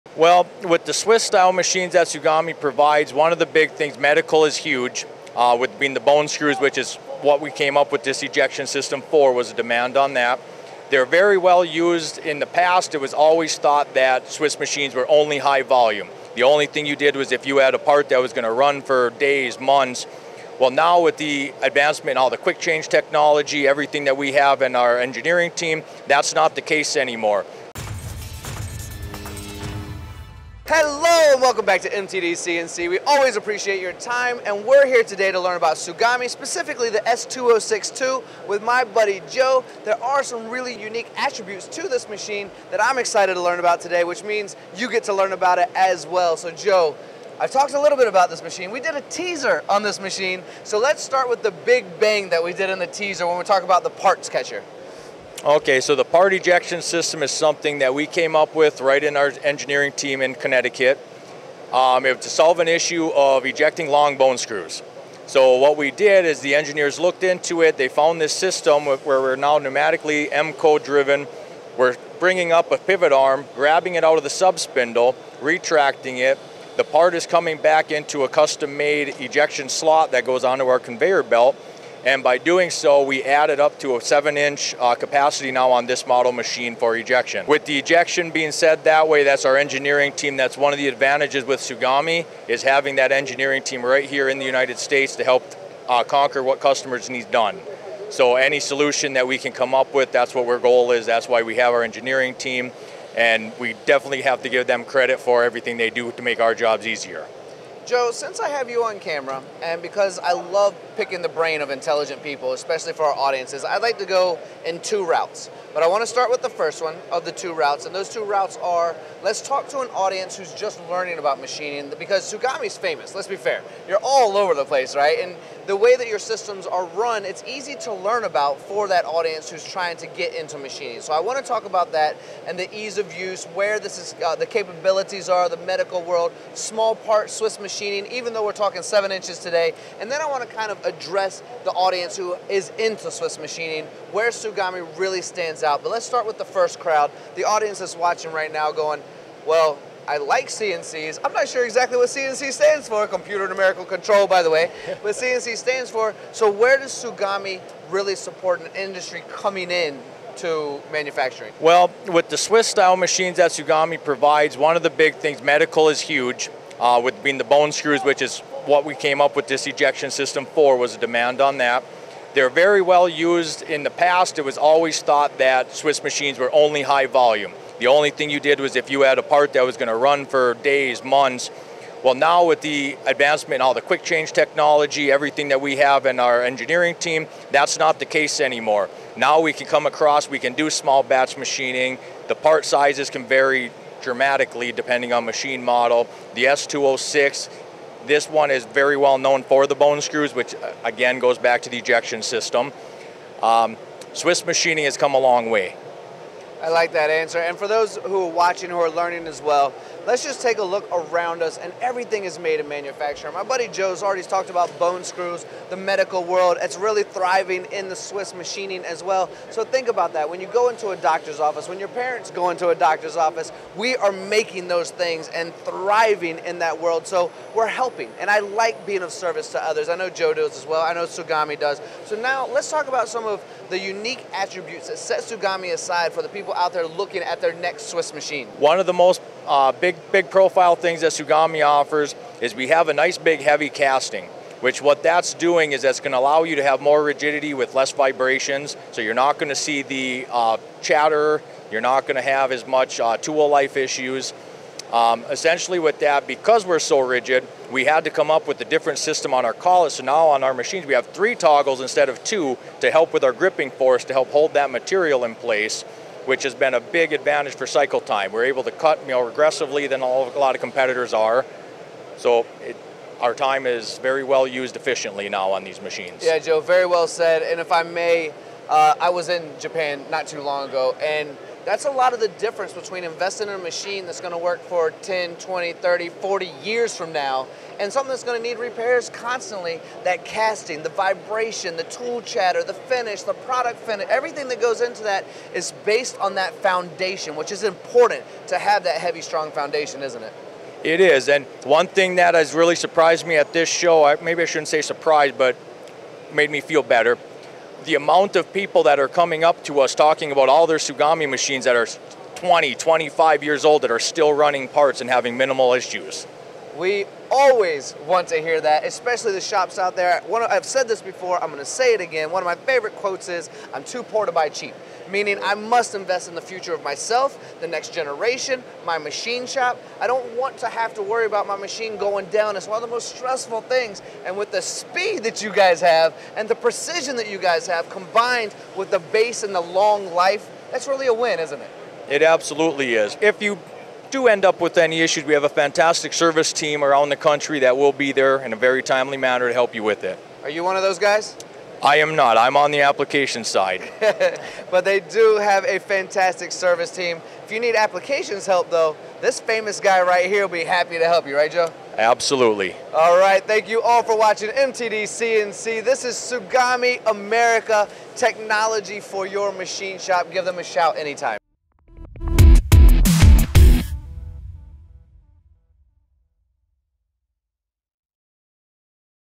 THIS is Engineering: Swiss-Style Mastery
Ever wondered how Swiss-machine engineering evolves to meet today's manufacturing demands? Dive into this exclusive MTDCNC North America interview with Joseph Dragisich from TSUGAMI AMERICA as we explore the S206-II - opposed gang tool CNC lathe. We're uncovering the brilliance behind Tsugumi America's custom-made parts ejection solution for long bone-screws, a game-changer for the medical industry. With advanced Quick Change technology, Tsugumi proves versatility isn't just for high volume production anymore. Learn about their commitment to reducing vibrations, enhancing tool life, and ensuring a sturdy, long-lasting machine. Whether you're new to CNC or a seasoned pro, this is the Swiss-machine insight you've been waiting for. Ready to elevate your manufacturing game? Tap into Tsugumi's American-engineered excellence.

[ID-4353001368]